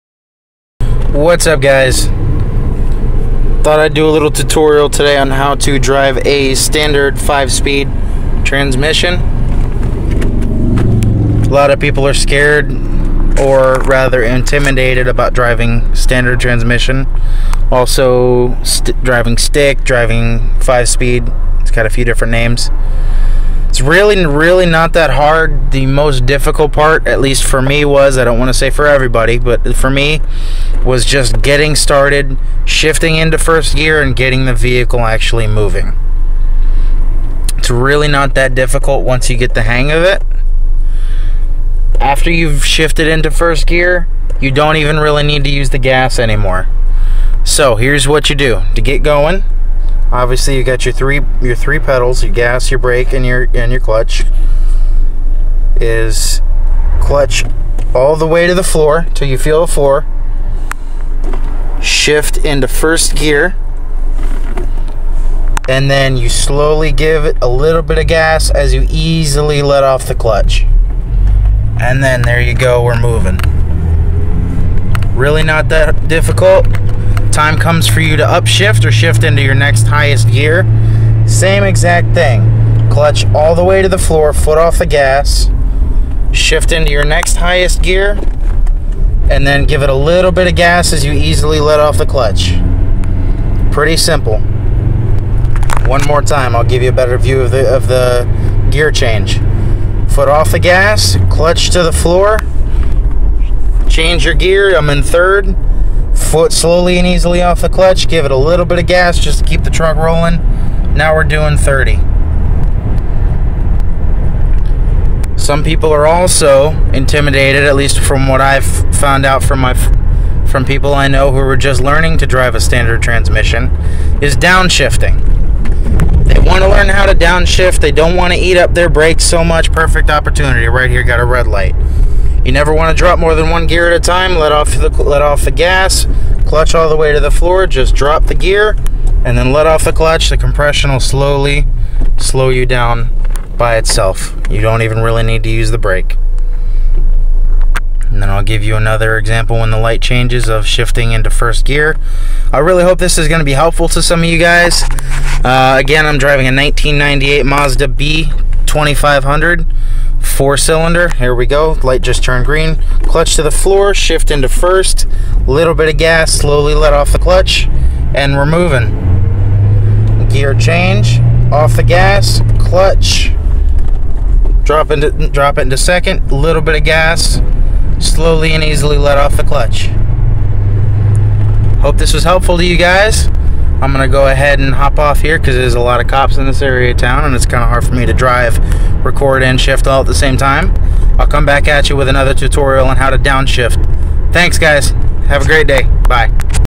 How to drive a 5-speed manual transmission
Quick tutorial on driving stick